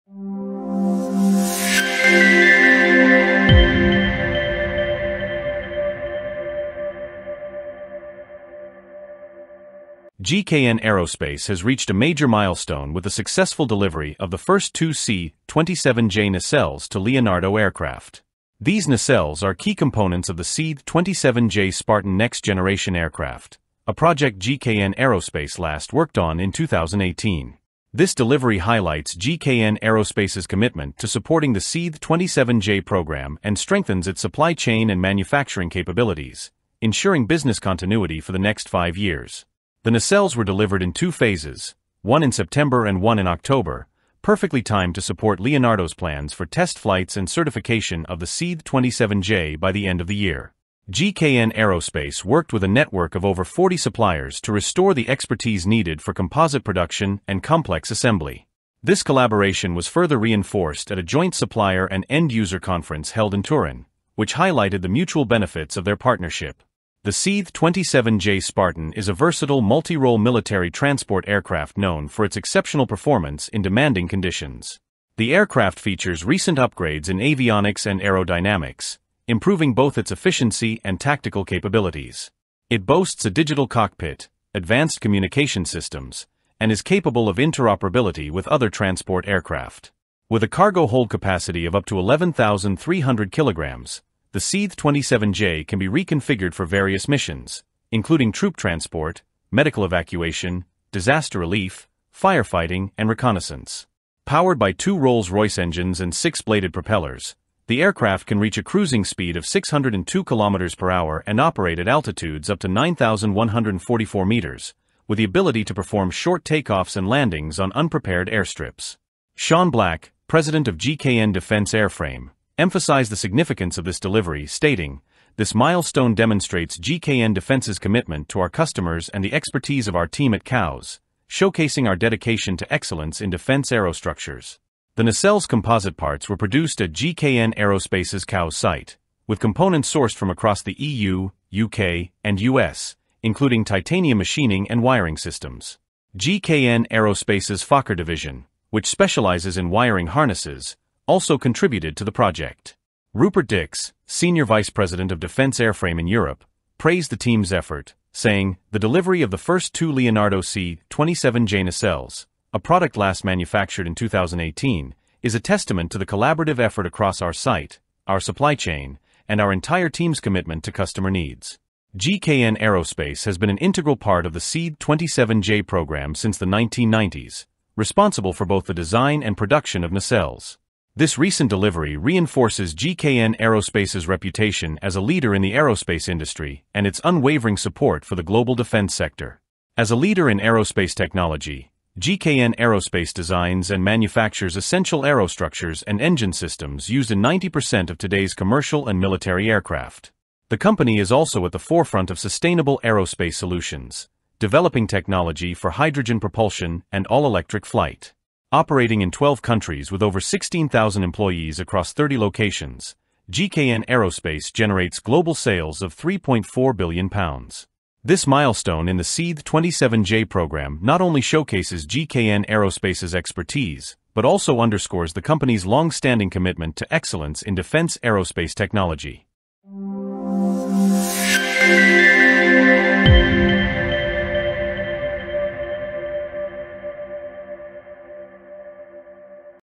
GKN Aerospace Revitalizes Supply Chain to Deliver First C 27J Multi Role Transport Aircraft Nacelles
GKN Aerospace has successfully delivered the first two C-27J nacelles to Leonardo Aircraft, marking a significant milestone in their partnership. This delivery supports the C-27J Spartan Next Generation program, enhancing its capabilities for military transport operations. The nacelles were produced with advanced composite materials and assembled with components from across the EU, UK, and US, demonstrating GKN’s global manufacturing strength. As part of its commitment to the defense aerospace industry, GKN Aerospace continues to lead in innovation, supporting critical programs with advanced aerostructures and sustainable technology. This achievement further strengthens the ongoing collaboration between GKN Aerospace and Leonardo, ensuring business continuity for years to come.